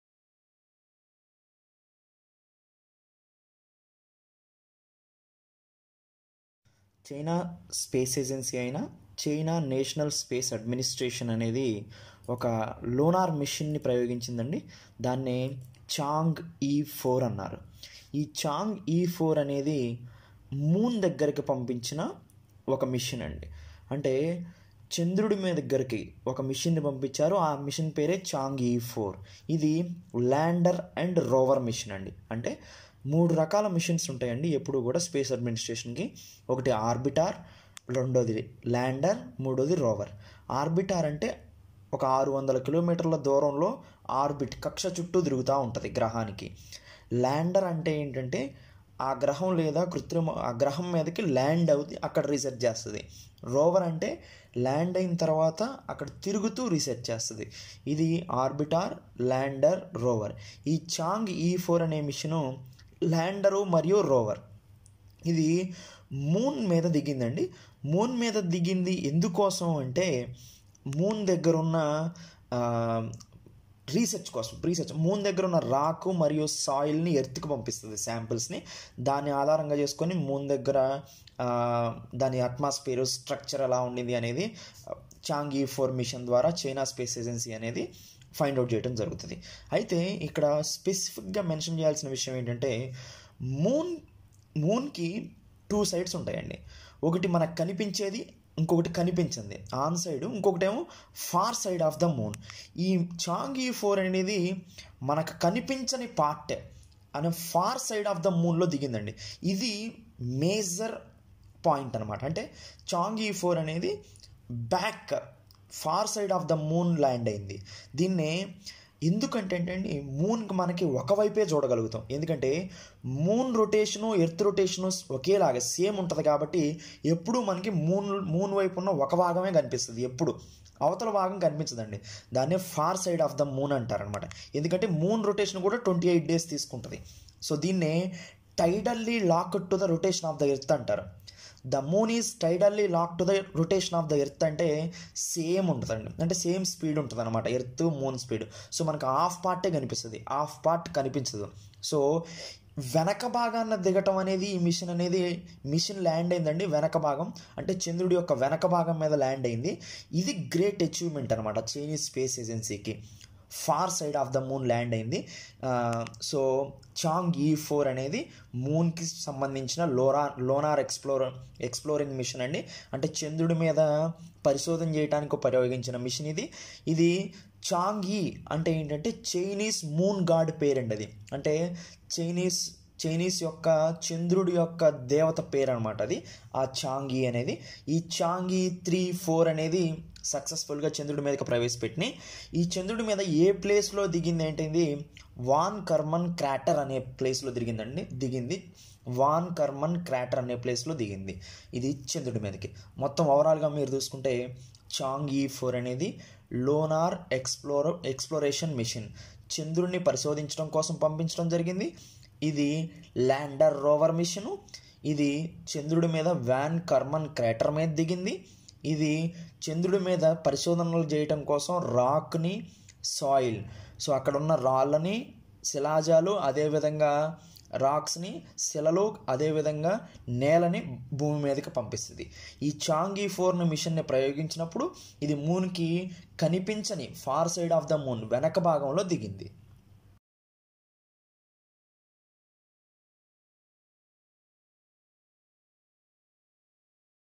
CURRENT AFFAIRS(Chang e-4 mission to moon - CHINA) BY SRAVAN MUTYALA IN SVR ACADEMY VISAKHAPATNAM
for online coaching